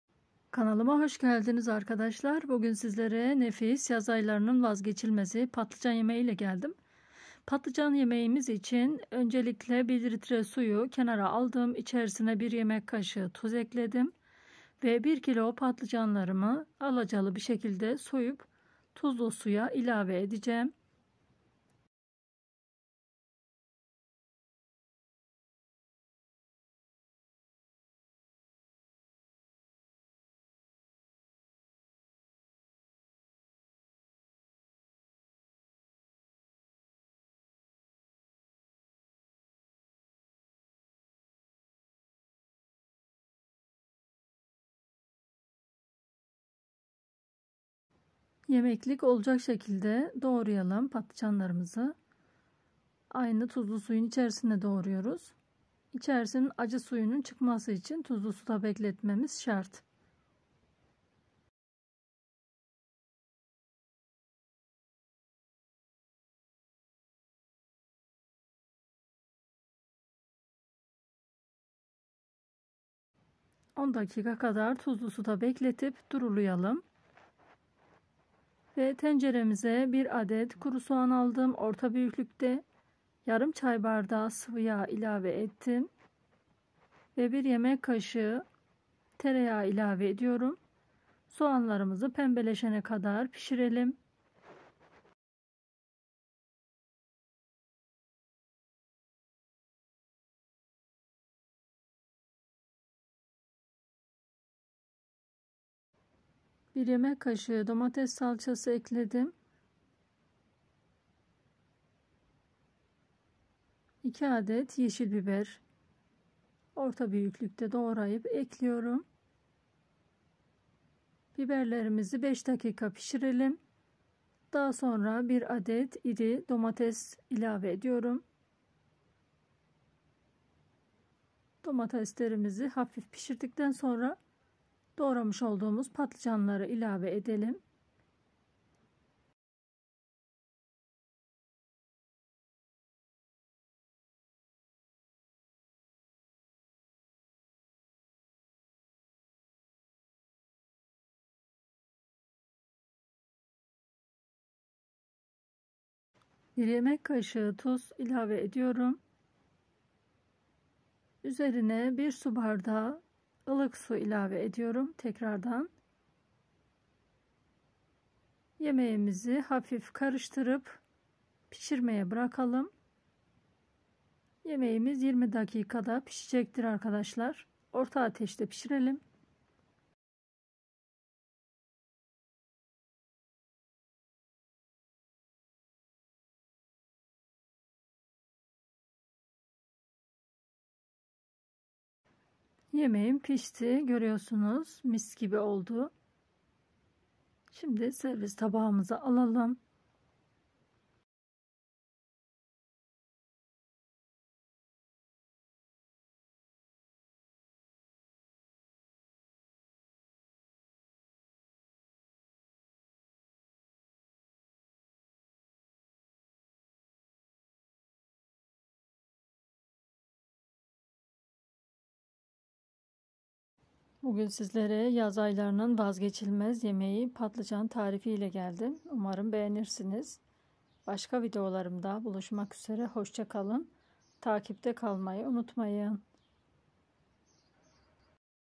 ETTEN DAHA LEZZETLİ SEVEREK YAPIP YEDİGİMİZ PATLICAN YEMEĞİ TARİFİ
Herkese selamlar saygılar sunuyorum arkadaşlar yaz kış demeden çok severek yapıp yediğimiz patlıcan tarifi ile geldim buyurun listemize

PATLICAN YEMEĞİ
bir kğ patlıcan
bir adet orta boy soğan
iki adet yeşil biber
bir adet domates
bir yemek kaşığı salça
yarım çay bardağı sıvıyağ
bir yemek kaşığı tereyağı
tuz